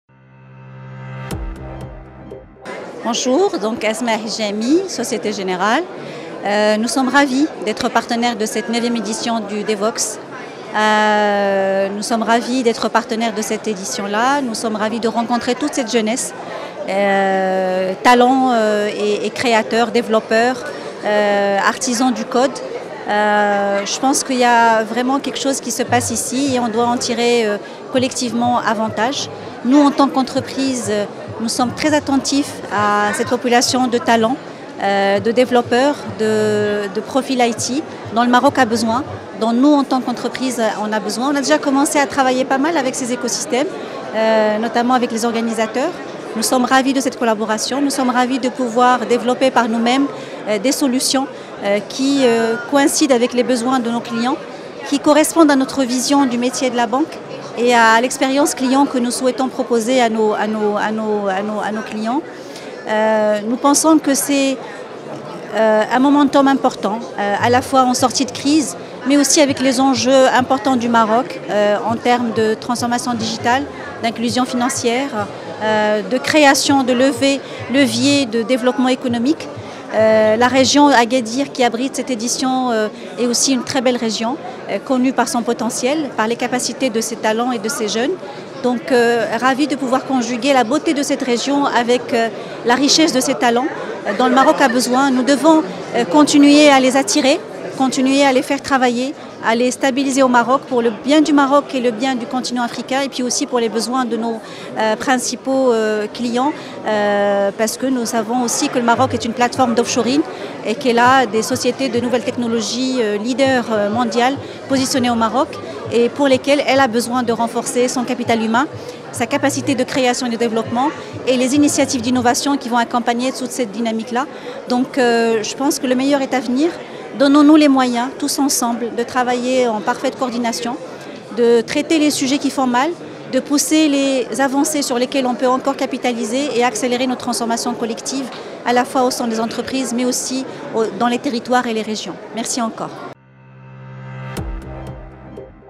Asmae Hajjami: "Le Maroc a besoin de talents IT"
Société générale Maroc est partenaire de la 9ème édition de Devoxx. Besoins de talents IT, accélération de la digitalisation...la directrice générale de la banque, Asmae Hajjami livre sa vision